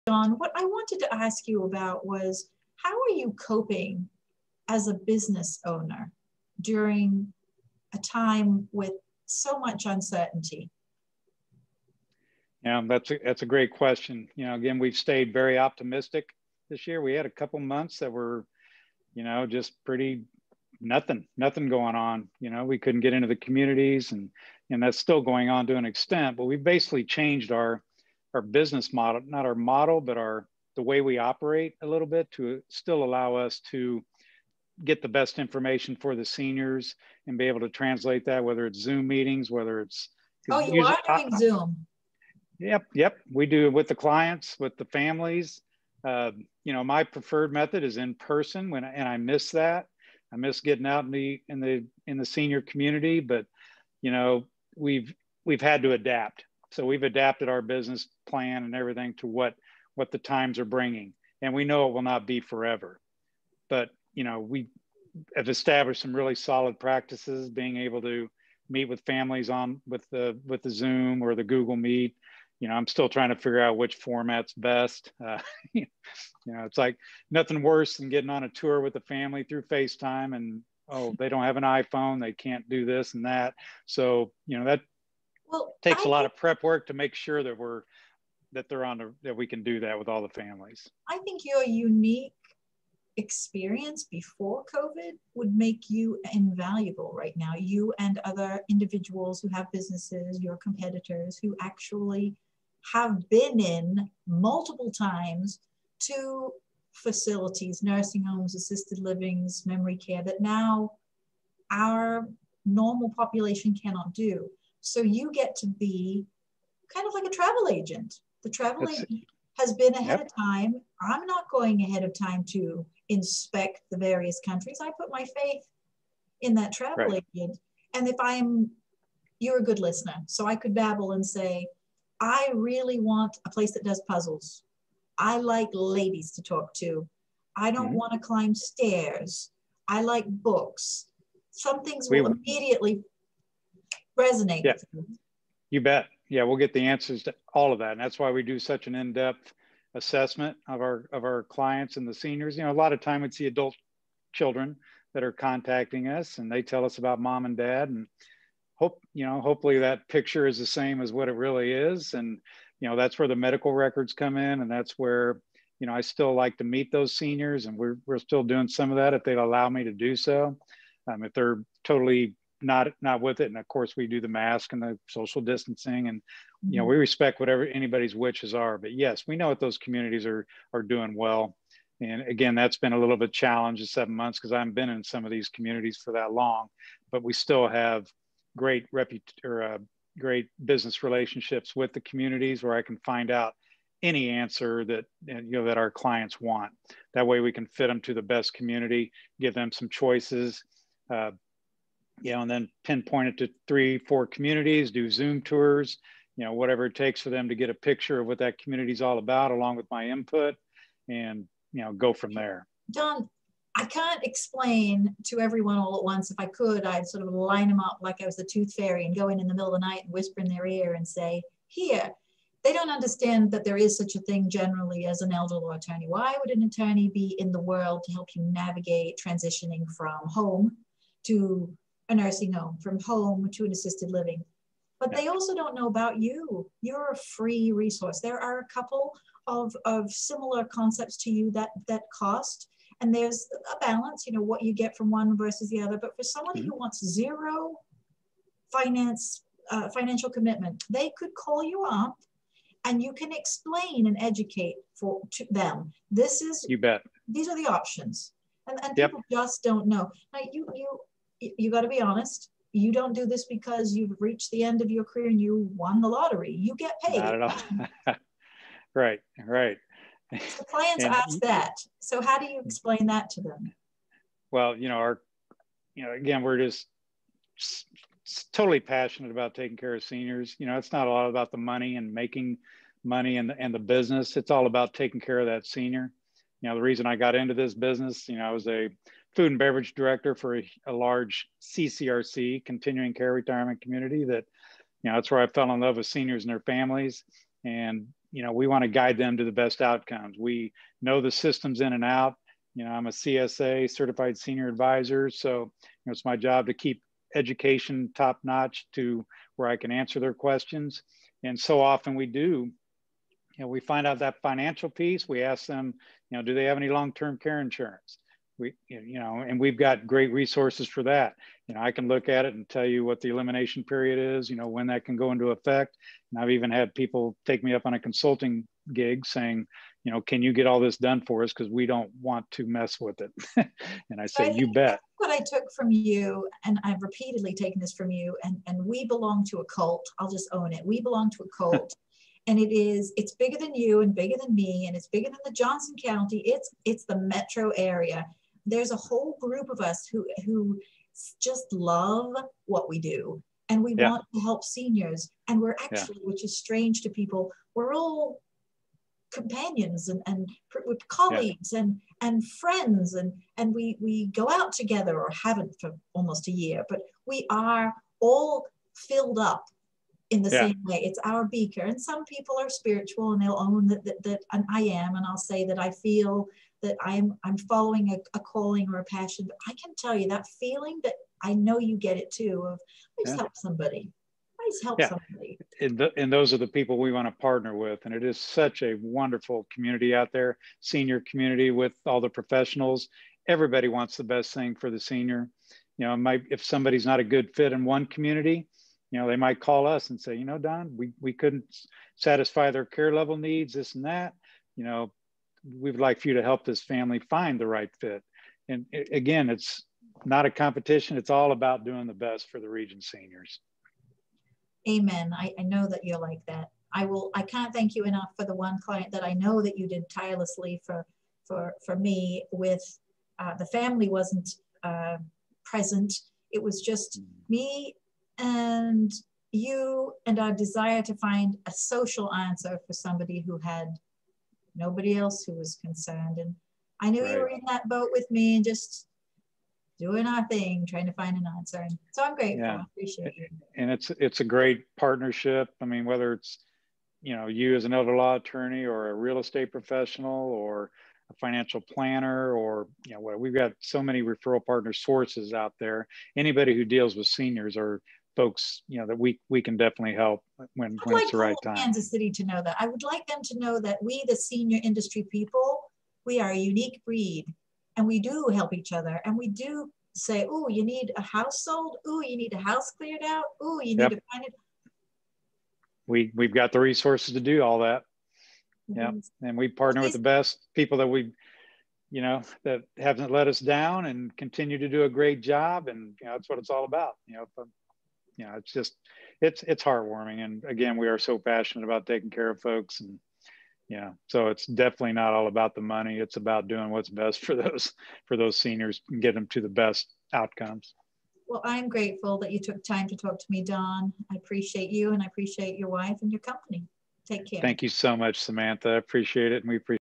Elder Care Video Series: Conversation with Don Minter Part 2
As we continue our chat, Don shares how he, as a business owner, is coping with the economic uncertainty many are experiencing as well as what he has done to still get the best and much needed information to seniors and their families.